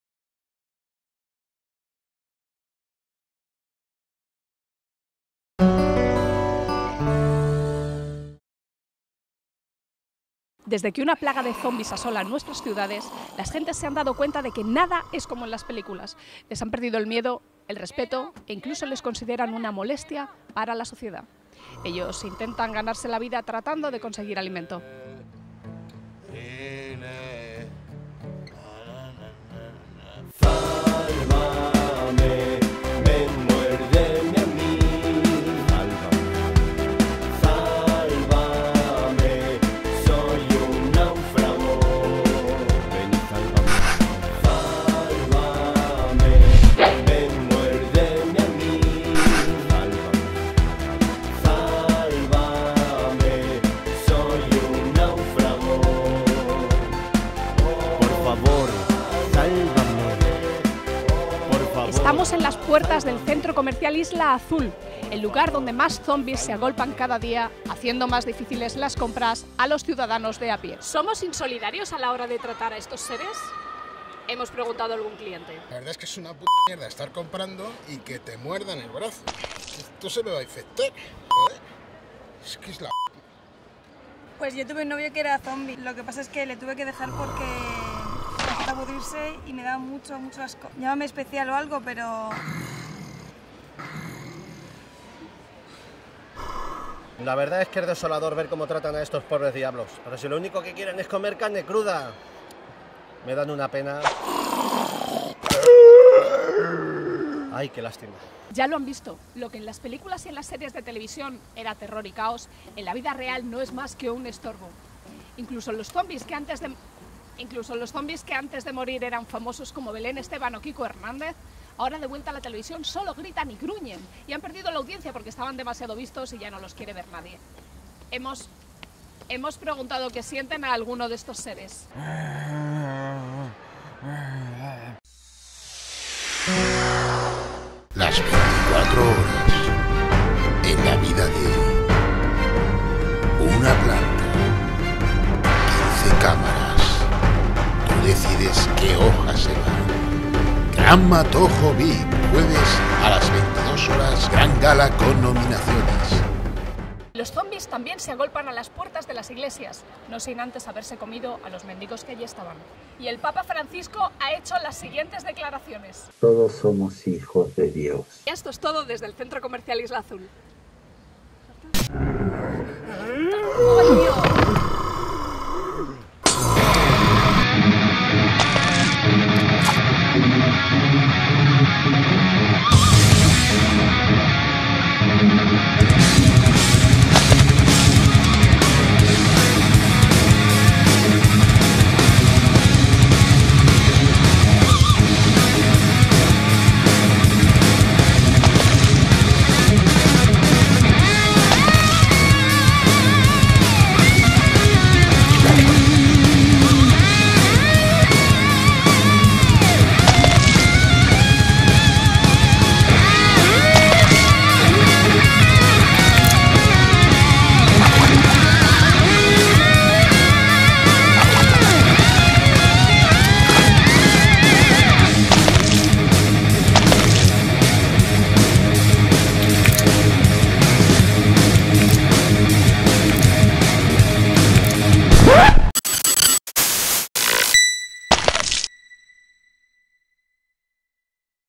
ZALVAME DEL ZOMBIE. ISLAZULTURA 2016
¿Son los zombies un peligro? La ciudadanía cree que no. Hacemos un reportaje en el centro comercial donde se agrupan algunos de estos seres molestos para ver qué opina la gente y sacar algo en claro de esta lacra social que nos ha invadido
Autor: Jeromin Cantero